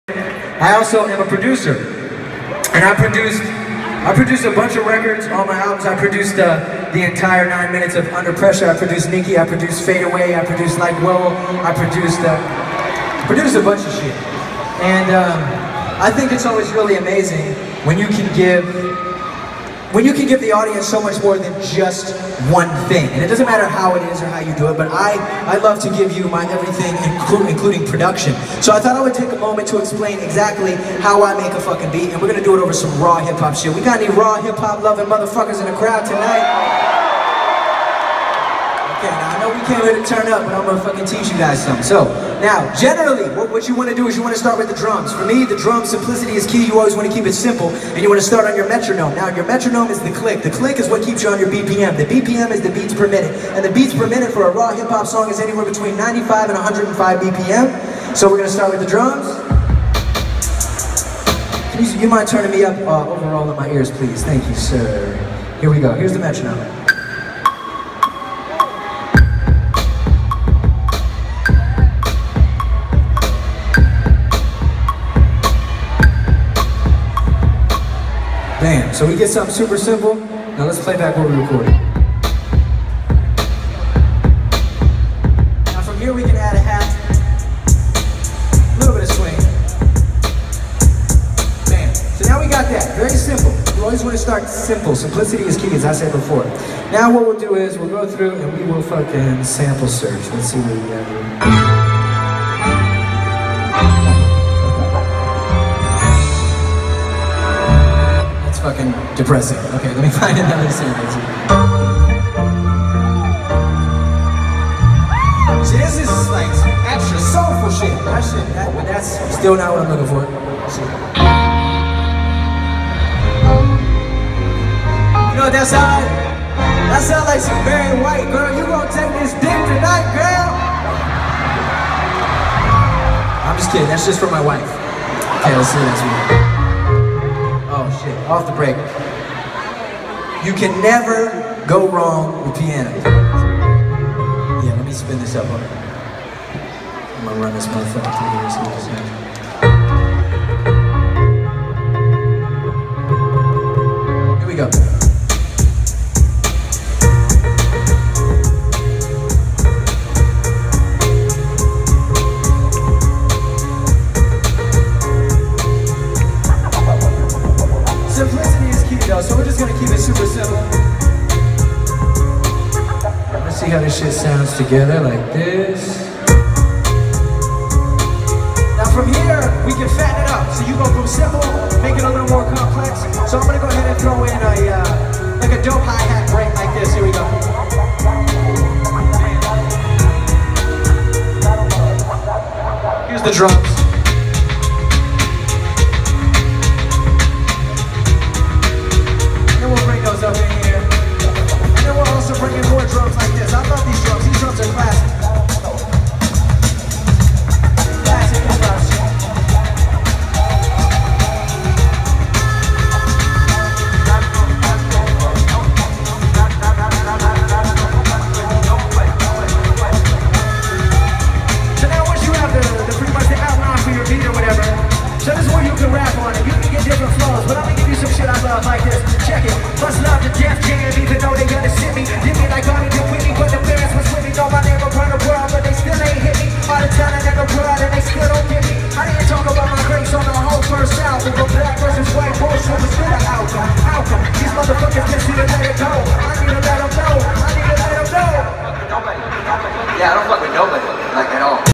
Logic Makes A Beat On Stage And Raps To It
Logic produces a beat on stage and raps to it

Visionary Music Group